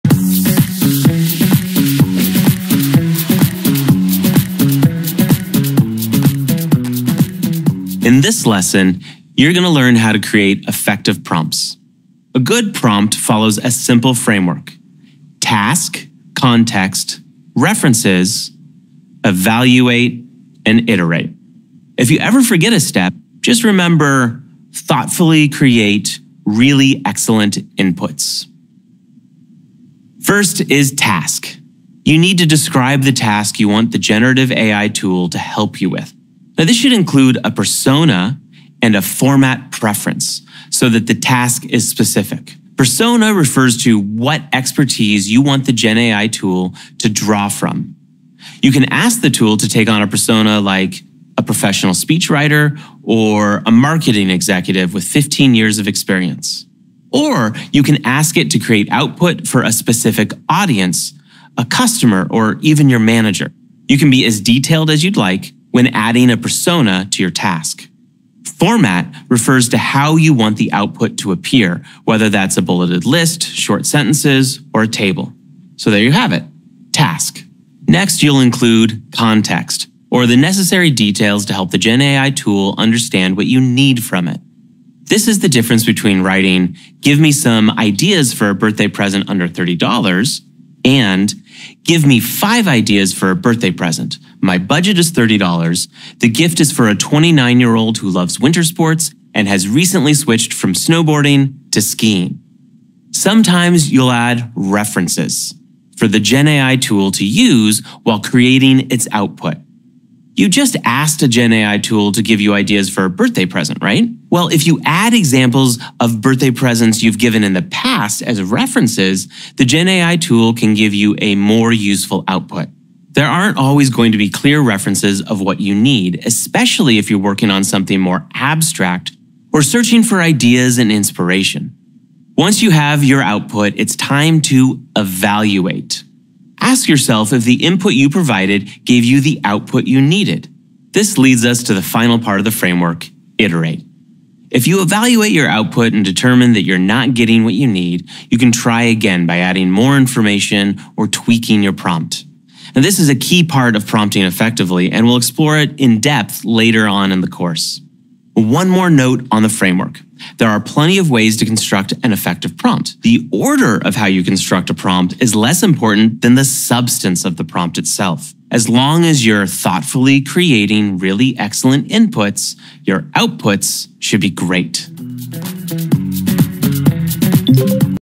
5 Bước Tạo Prompt AI Hiệu Quả Cho Người Mới
Học cách tạo prompt AI hiệu quả với khung TCARE:

Task (nhiệm vụ), Context (ngữ cảnh), References (tham khảo), Evaluate (đánh giá), Iterate (lặp lại).

Video này giúp bạn hiểu cách xác định nhiệm vụ với persona và định dạng phù hợp, cung cấp ngữ cảnh chi tiết, thêm tham khảo hữu ích, đánh giá kết quả và cải thiện prompt.